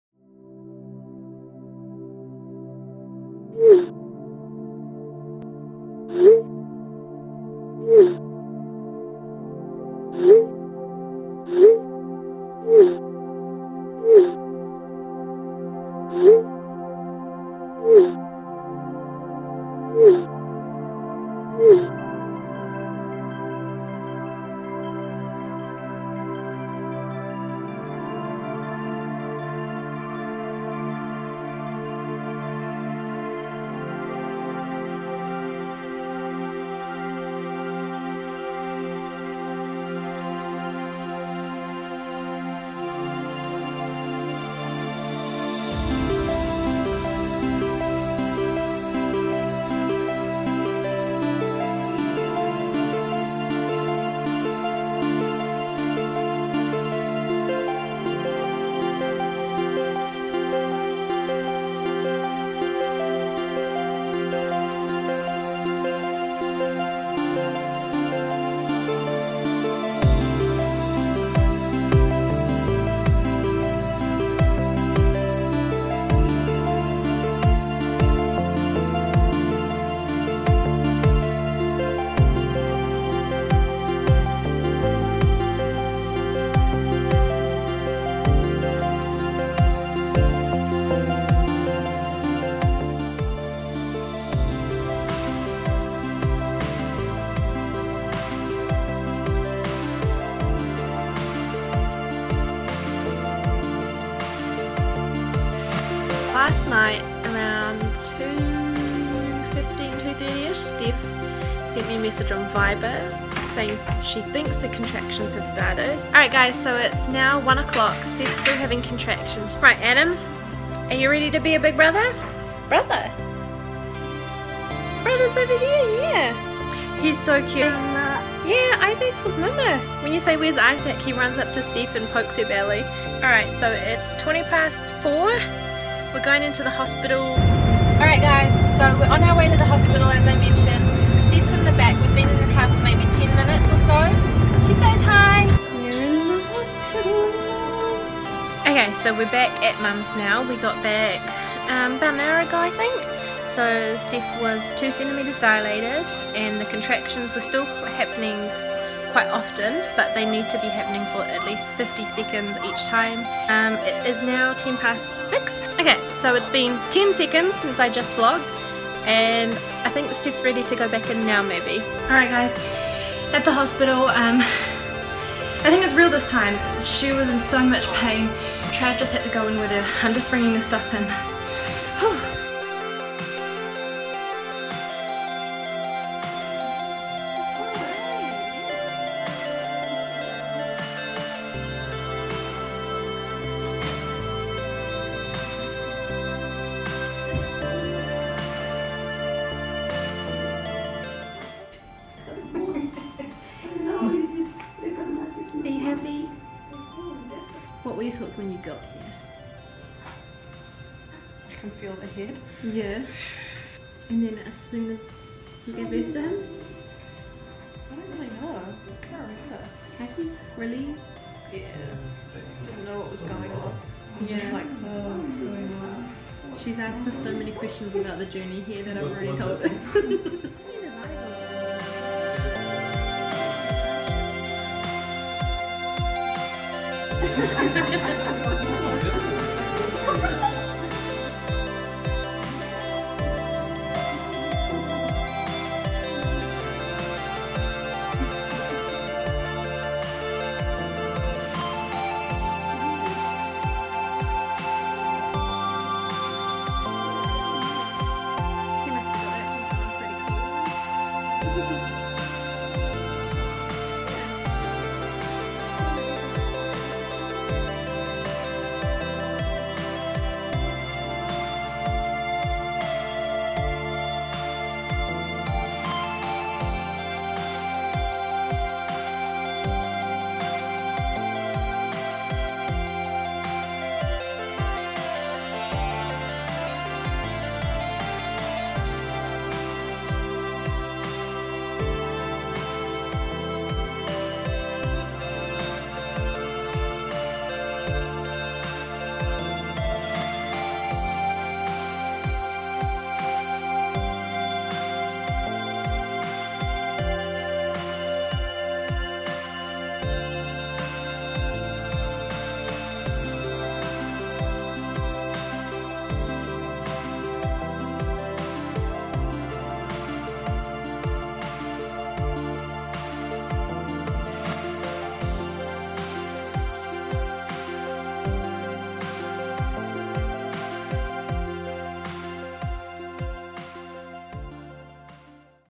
Welcome Baby Isaac - 7th Feb, 2016 || JessChillinAbout
Here's a video I made for Steph and to introduce little Isaacs journey into this world. Hope you enjoy x

As always thanks so much for checking out my channel and this video!

Jess :)